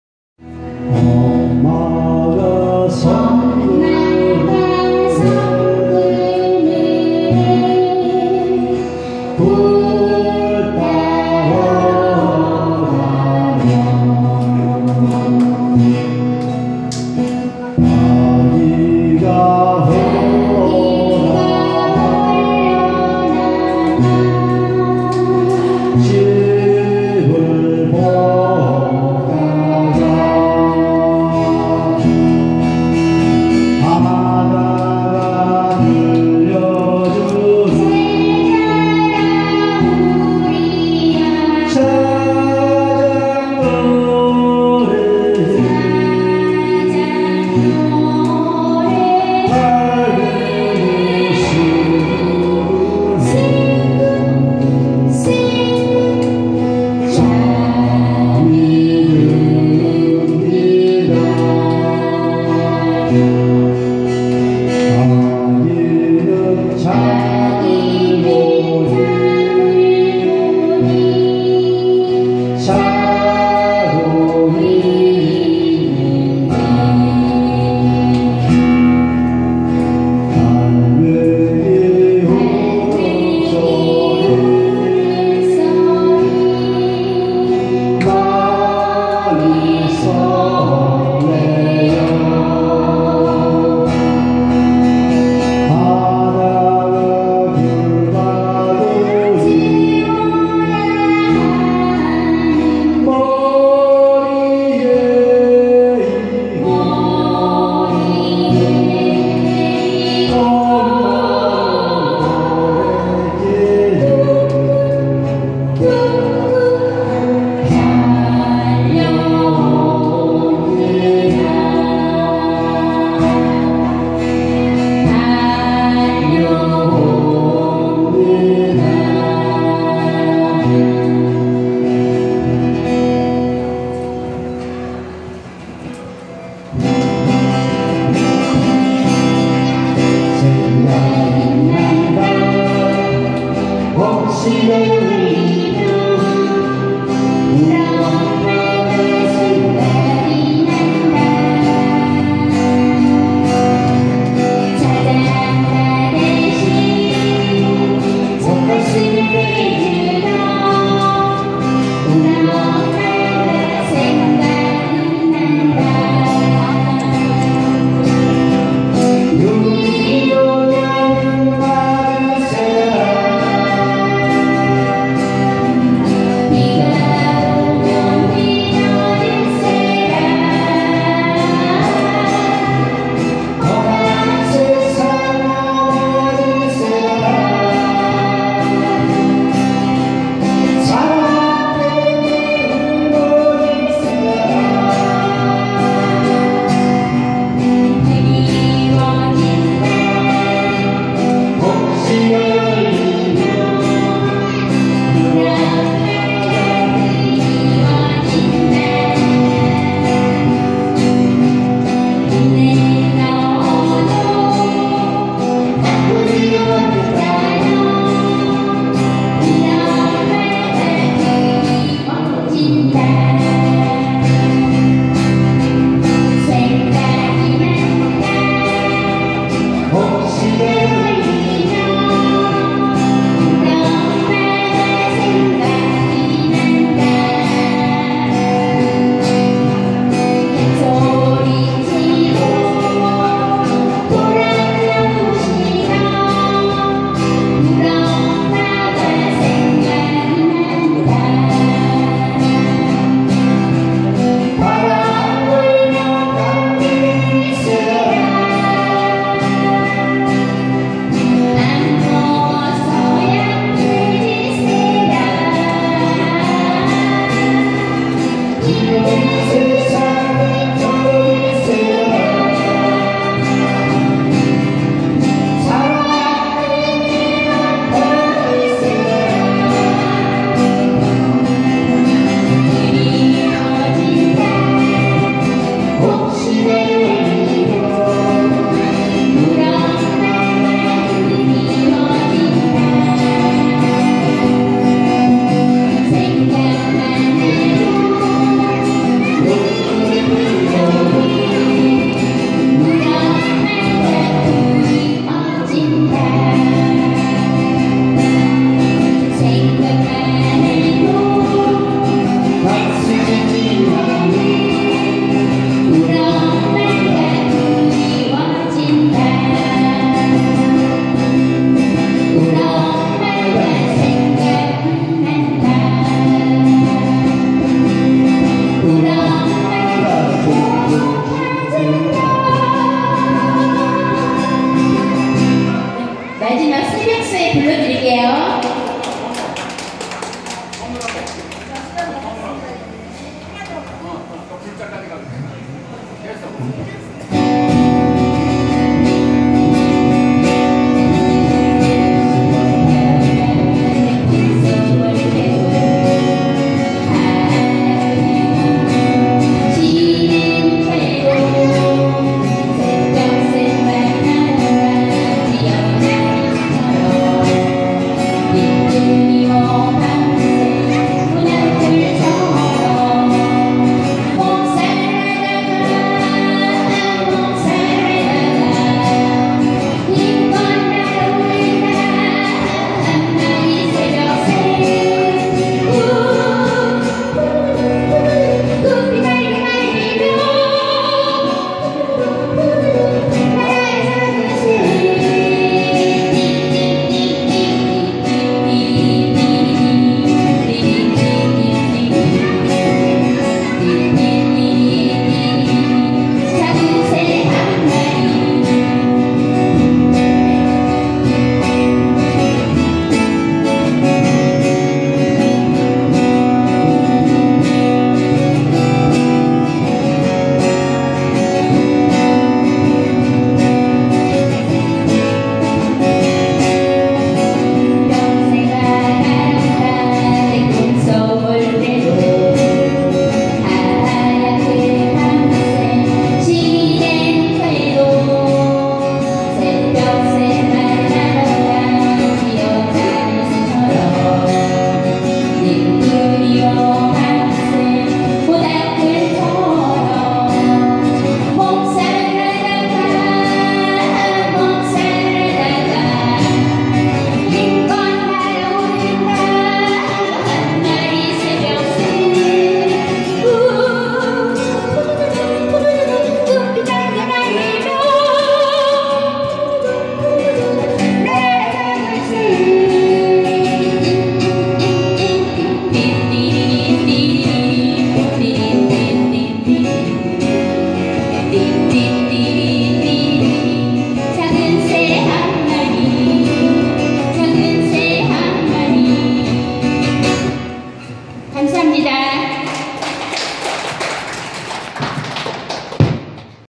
무궁화꽃예술단.제222회공연 굴포천역사.한상욱.임향숙
무궁화꽃예술단은 각분야에서.활동하시는 분들로 자진구성된.단체입니다..제222회공연 굴포천역사 오후3시 통기타.한상욱.
젬베가수.임향숙